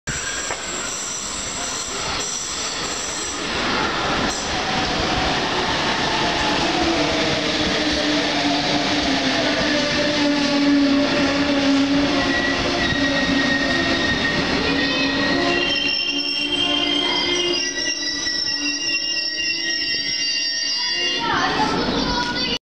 Metronom nach Lüneburg Einfahrt Hamburg-Harburg
Ein Metronom des Elbe-Taktes fuhr als RB3 in Hamurg-Harburg ein, auf dem Weg nach Lüneburg